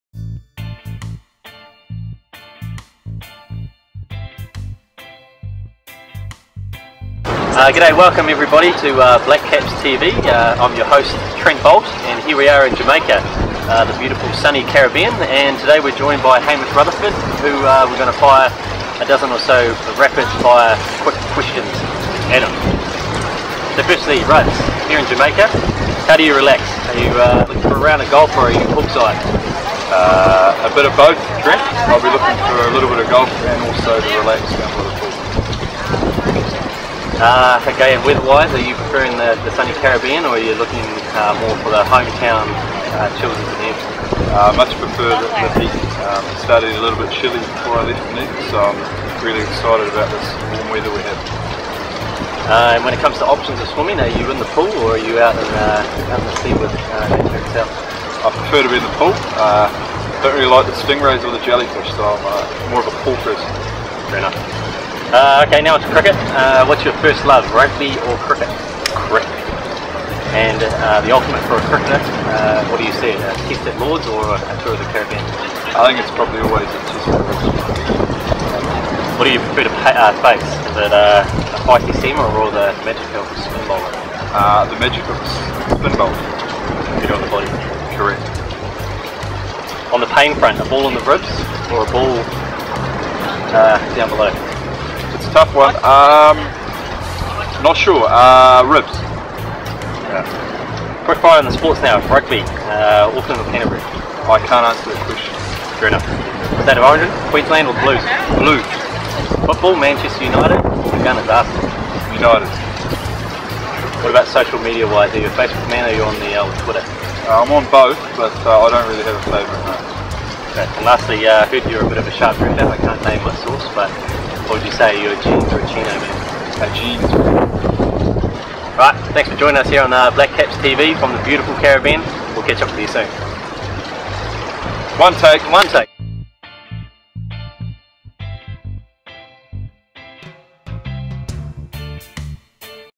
12 questions with Hamish Rutherford
BLACKCAPS TV reporter Trent Boult gives Hamish Rutherford a grilling on the eve of the BLACKCAPS West Indies tour.